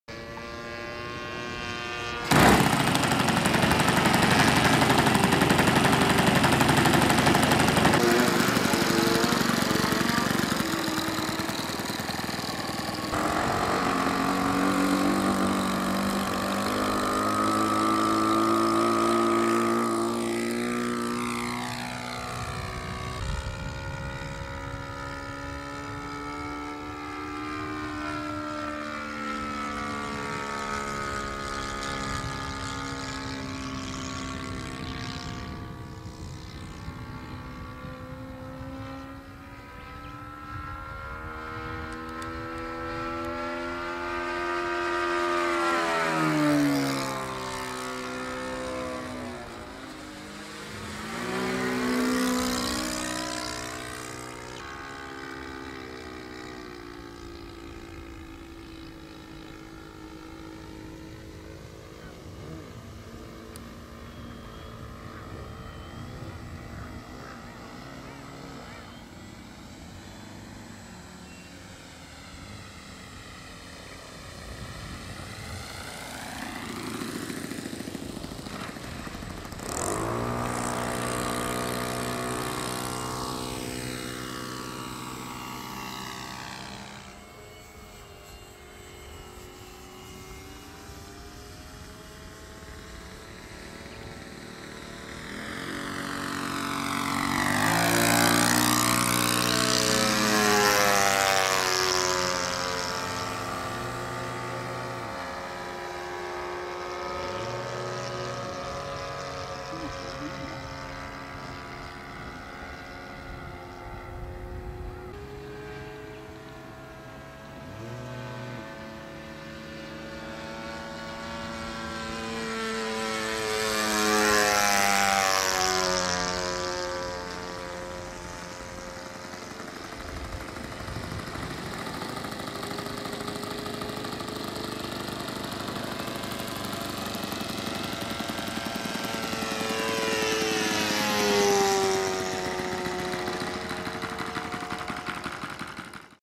'Calmato' Floatplane 水上飛行機「カルマート」 千葉フライングクラブ 2022/4/13
YS FZ110を搭載。Nさんが水上機の「カルマートアルファ60スポーツ」をフライト。